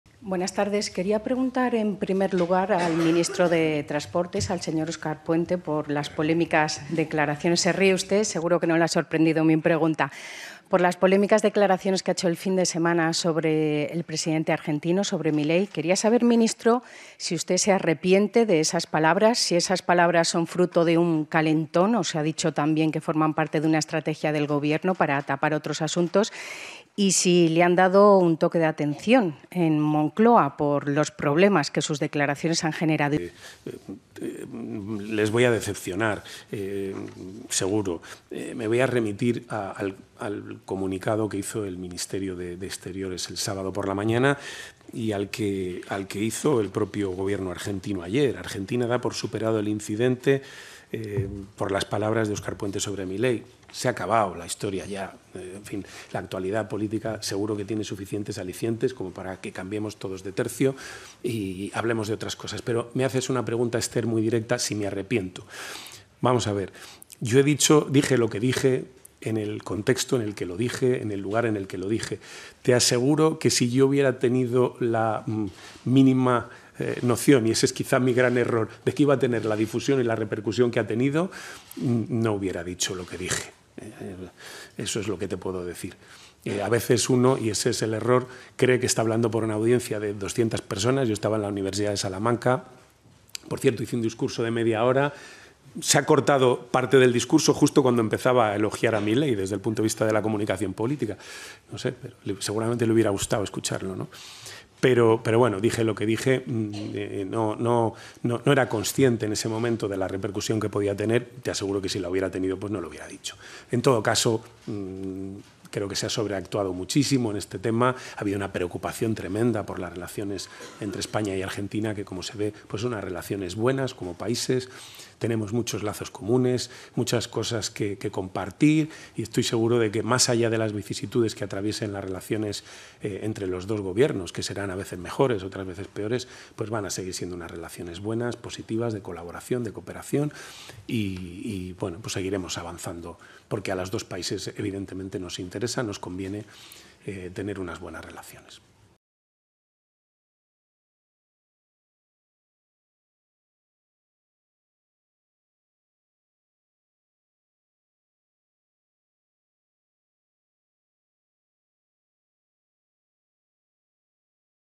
Óscar Puente rebaja a un hecho «anecdótico» su polémica con Milei: «No hubiera dicho lo que dije»
El ministro de Transportes, Óscar Puente, ha rebajado a un hecho «anecdótico» el conflicto con Argentina de este fin de semana después de sus insidias al presidente de aquel país, Javier Milei, del que insinuó que había consumido drogas antes de una entrevista en televisión. Lo ha hecho en la rueda de prensa posterior a la reunión del Consejo de Ministros, en la que ha acompañado a la portavoz del Ejecutivo, Pilar Alegría, y en la que también han comparecido la ministra de Ciencia, Diana Morant, y el titular de la cartera de Política Territorial y Memoria Democrática, Ángel Víctor Torres.

Pese a esa consideración inicial, Puente no ha pedido perdón, pero sí ha señalado que «si yo hubiera tenido la mínima noción, y ese es quizás mi gran error, de la difusión y la repercusión que ha tenido, no hubiera dicho lo que dije. A veces uno cree que está hablando para una audiencia de doscientas personas», ha concluido. El ministro se ha remitido a los comunicados de los dos gobiernos después de sus palabras, para dar por superado el choque diplomático.

Pese a esas consideración inicial, Puente no ha pedido perdón, pero sí ha señalado que «si hubiera sido consciente de la repercusión que ha tenido no lo hubiera dicho».

En tono conciliador, el exalcalde de Valladolid se ha referido al contexto y el lugar en el que pronunció sus palabras, durante una charla en la Universidad de Salamanca con estudiantes de Ciencias Políticas, a los que recomendó que si hacen carrera política se comporten «tal y como sois». Su intervención ha hecho poner el grito en el cielo a muchas personas de la carrera diplomática consultadas por ABC, que consideran incluso que el ministro tenía que haber sido cesado.

Puente, además, ha dicho que en los estractos que más se han difundido de su charla no se incluye uno en el que «elogiaba a Milei, desde el punto de vista de su la comunicación política. Seguro que le hubiera gustado escucharlo» ha indicado con media sonrisa.

Pese a todo, Puente ha asegurado que «se ha sobreactuado muchísimo en este tema», y ha abogado por que las relaciones bilaterales con Argentina sigan siendo «buenas, positivas, de colaboración», ya que «tenemos muchos lazos comunes, y muchas cosas que compartir».